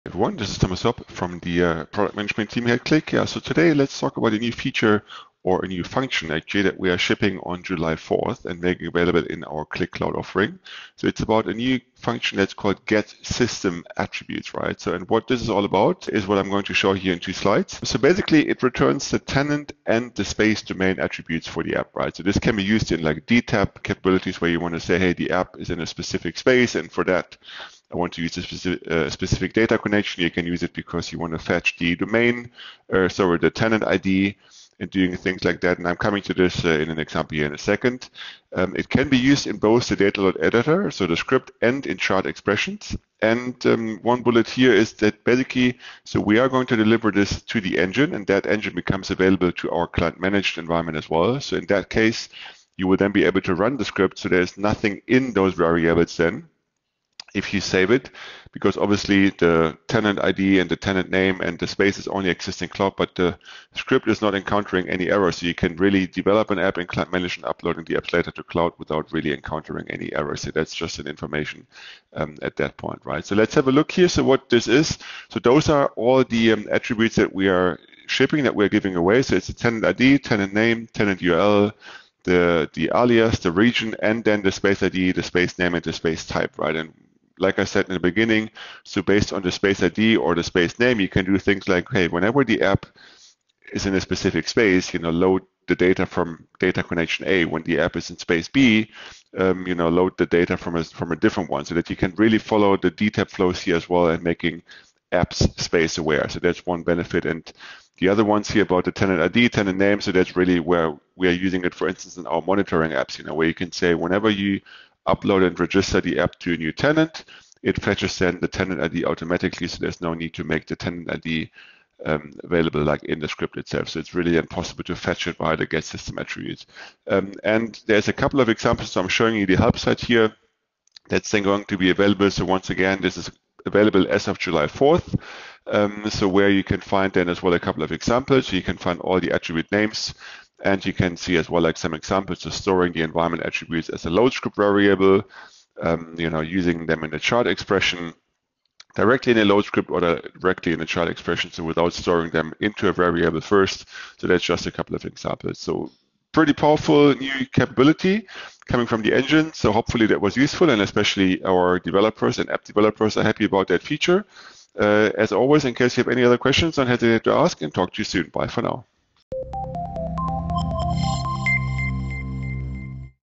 GetSysAttr - script and chart function
We’ve added a new script function that makes it easier to leverage variables for enhanced monitoring across apps and tenants without the need to manually enter Tenant IDs, Tenant Names, or the URLs into the script.

For the programmatic deployments, the spaceId and spaceName are also important.

The new script and expression function:
Dual GetSysAttr(VariableName)

Variables accessible in this scope:
- tenantId
- tenantName
- tenantUrl
- tenantUrlAlias
- tenantRegion
- spaceId
- spaceName
- spaceType